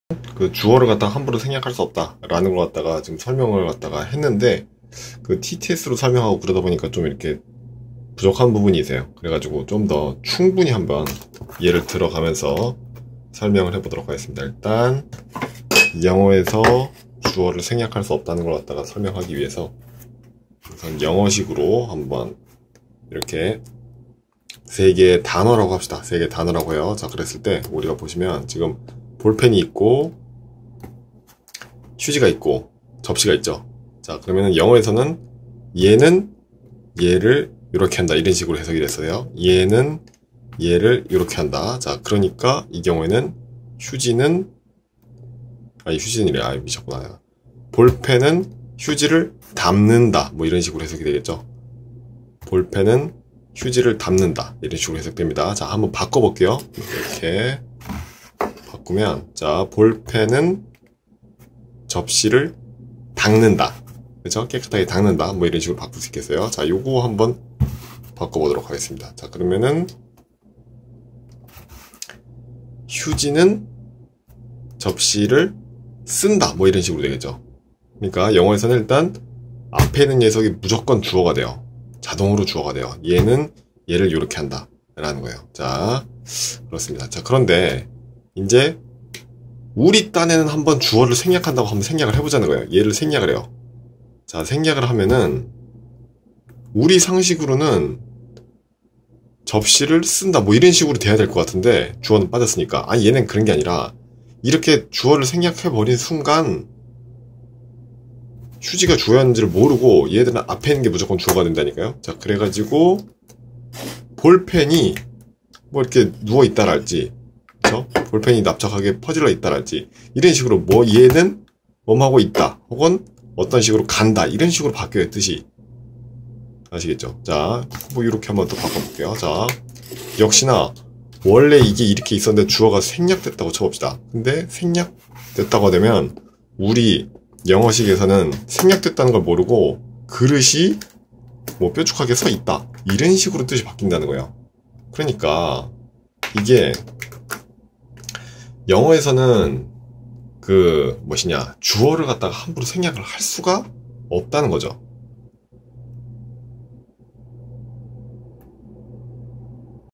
영어에서 주어를 함부로 생략하지 못하는 이유
CC0입니다. 저작자 표시 없이 마음껏 쓰셔도 됩니다.

구와 절 설명의 추가 설명입니다.

TTS로 만드는 것은 몇 일 걸렸는데,
이거는 그냥 휴대폰으로 바로 찍은 것입니다. ㅋ